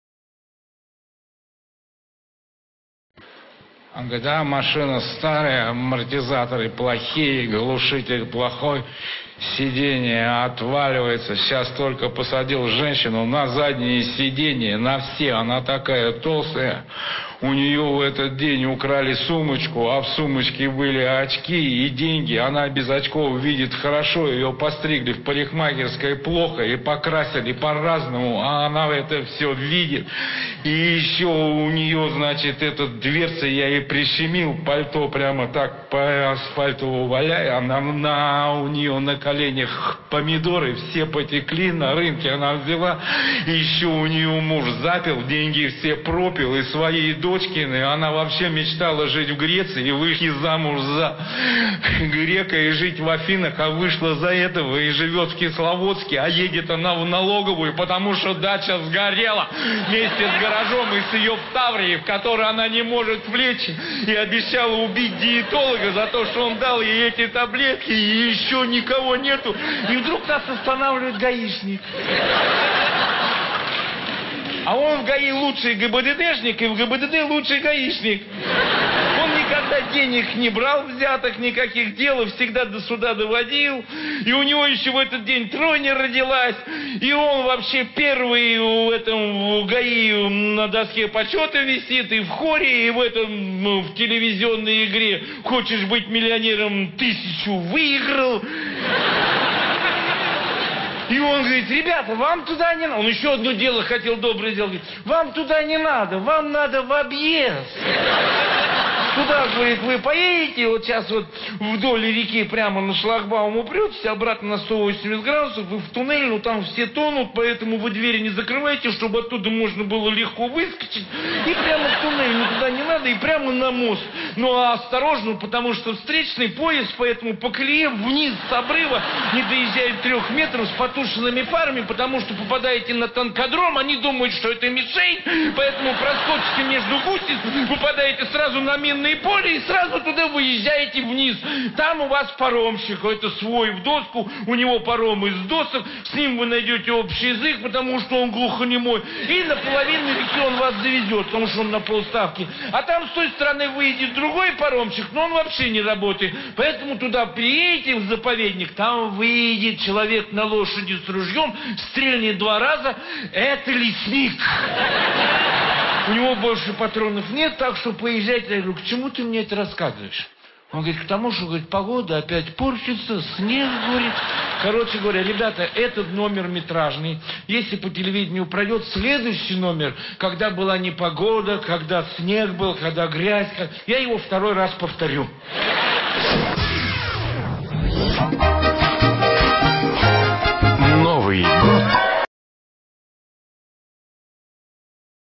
Ян АРЛАЗОРОВ НЕСТЫКОВОЧКА 100% СМЕХ ДО СЛЕЗ!!
Ян Арлазоров (Yan Arlazorov) выдающийся юморист, который навсегда останется одним из самых ярких представителей в истории своего жанра. Он умел смешить и умел смеяться. Выступления Арлазорова основывались на тесном контакте с публикой.  Стараясь совершить больше, Ян постоянно гастролировал, придумывал новые сценки и представления